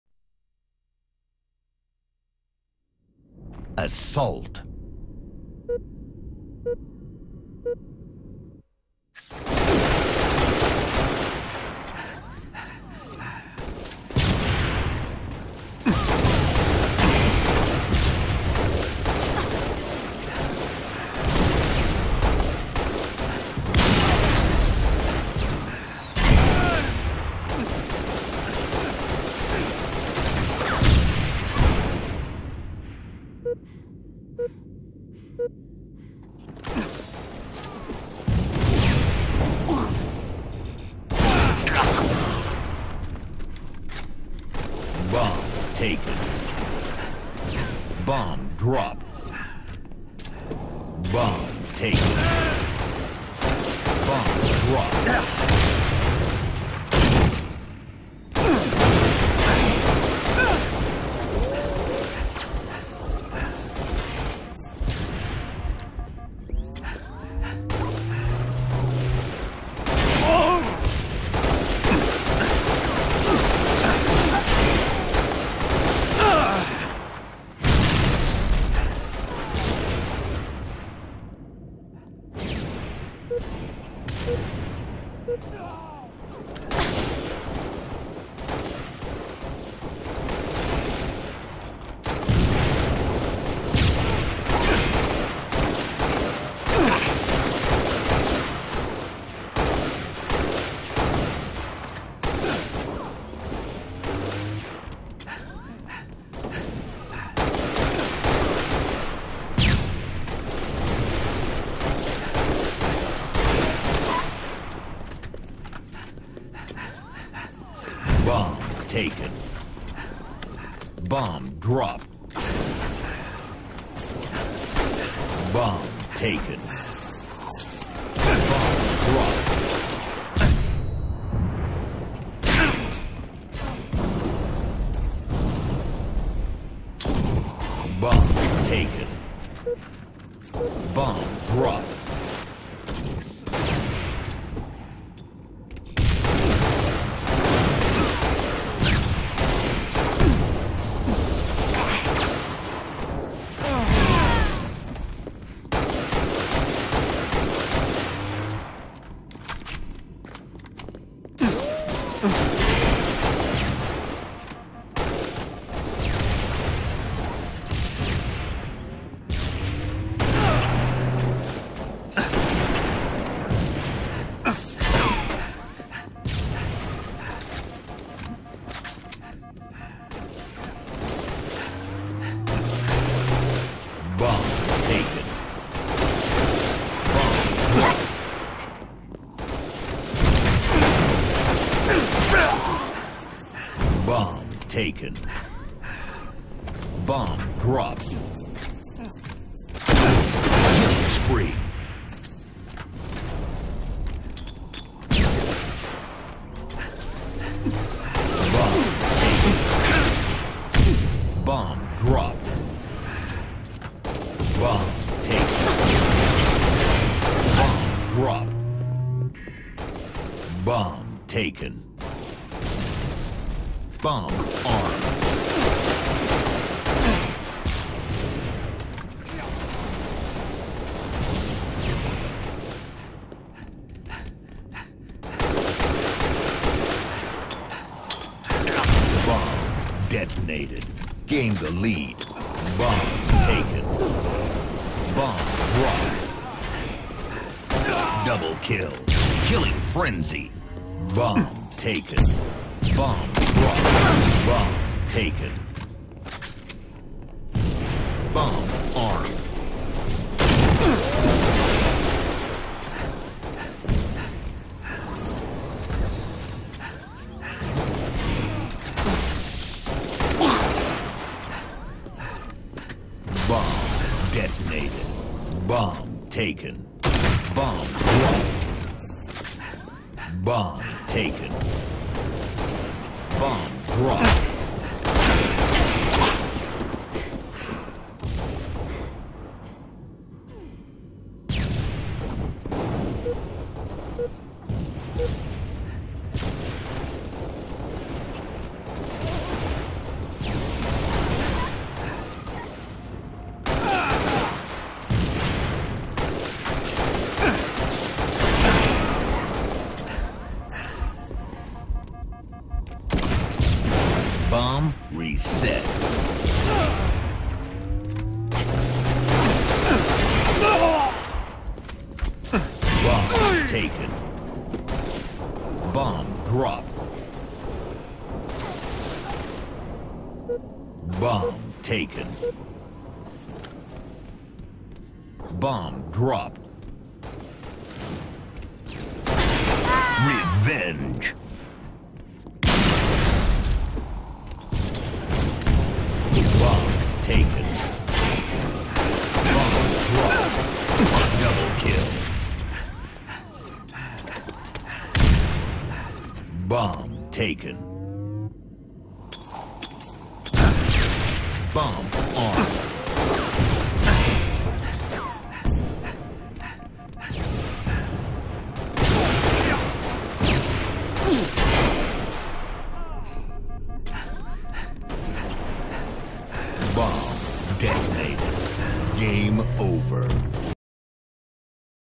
TSW vs 4G, Zealot Bomb
[Bald] Scrim gameplay against Team FourG. Captured by William101.